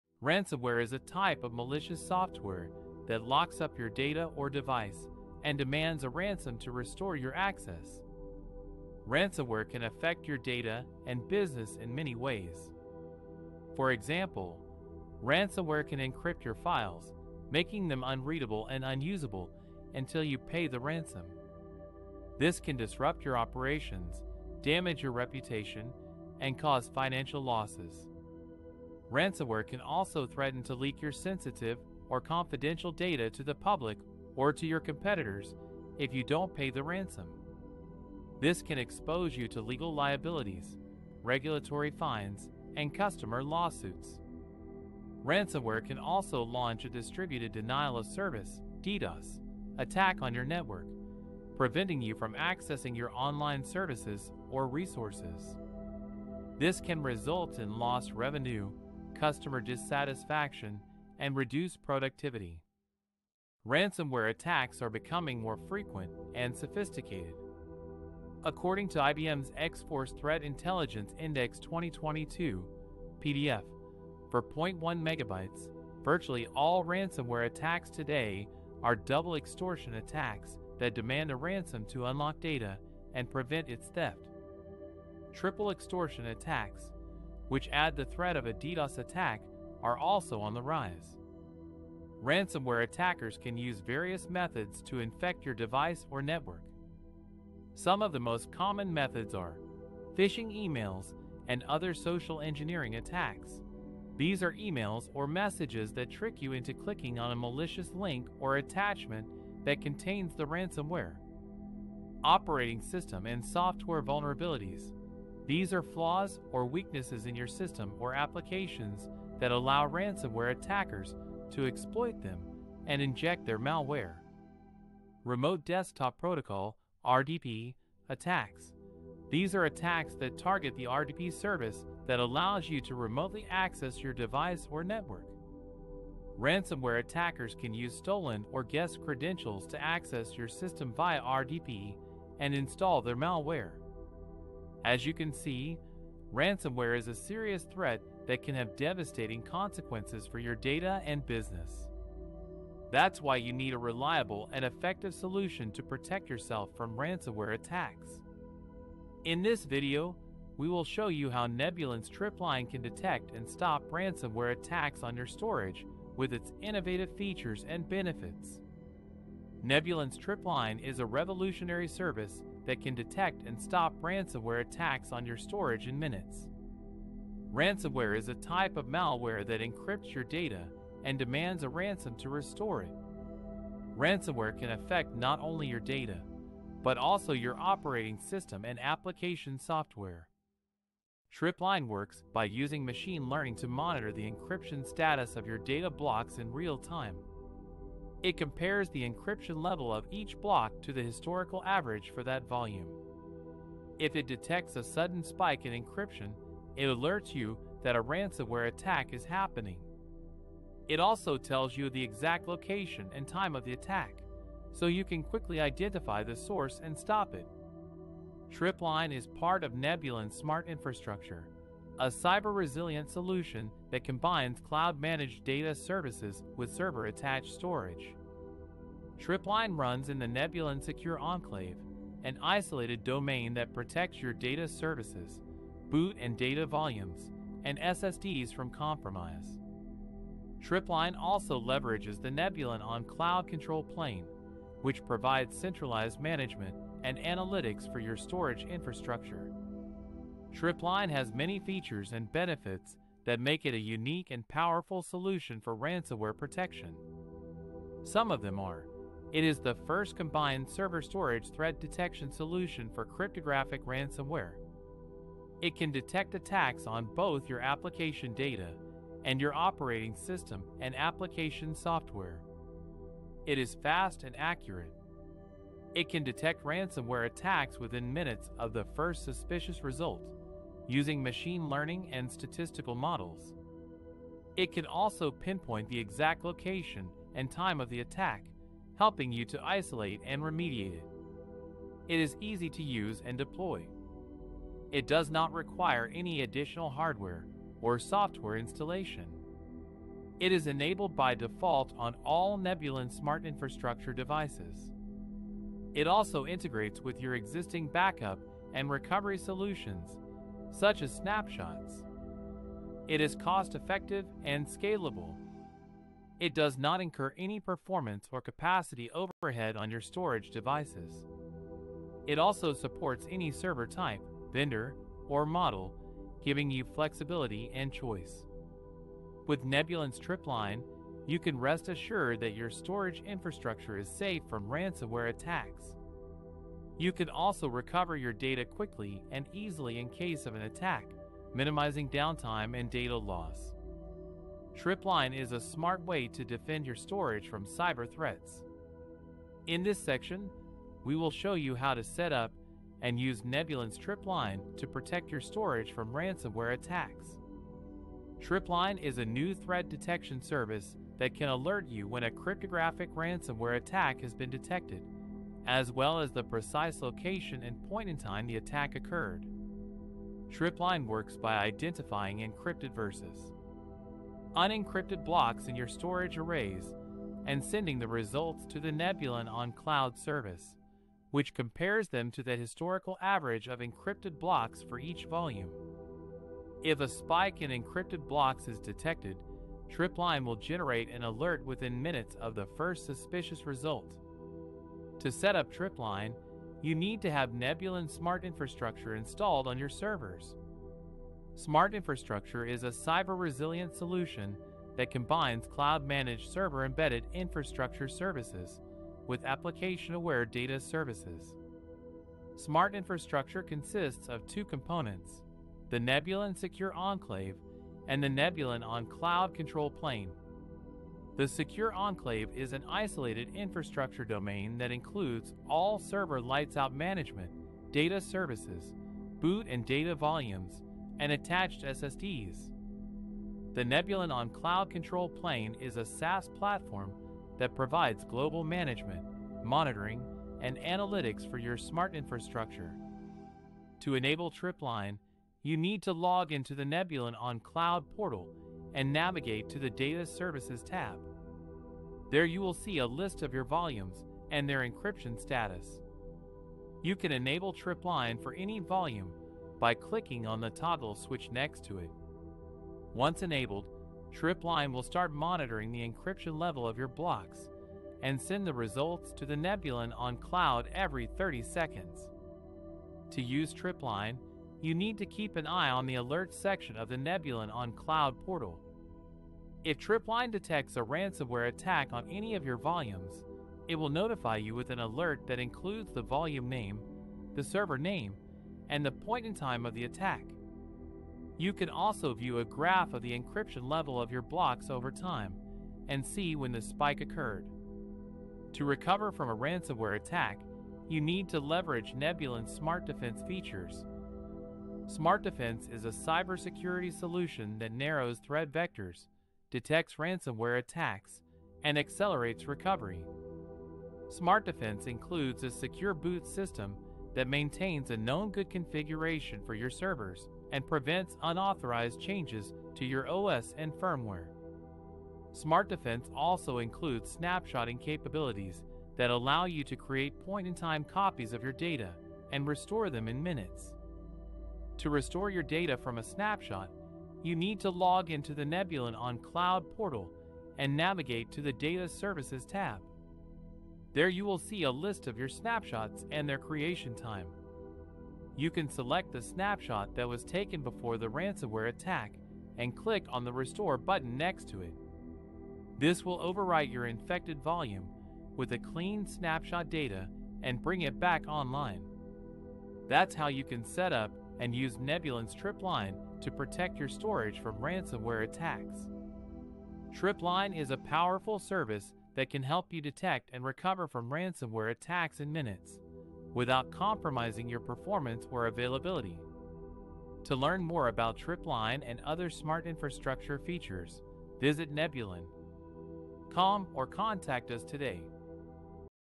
How Nebulon's Tripline Can Detect and Stop Ransomware Attacks on Your Storage
In this video, we will discuss how Nebulon's Tripline can detect and stop ransomware attacks on your storage. Nebulon TripLine is the first combined server and storage threat detection solution for cryptographic ransomware. It can identify attacks on application data as well as the operating system and application software. With Tripline, you can detect data being encrypted for the OS & data, pinpoint the time of attack, limit the spread & rollback with greater precision and recover in minutes. Join us to learn more about how you can protect your storage from ransomware attacks.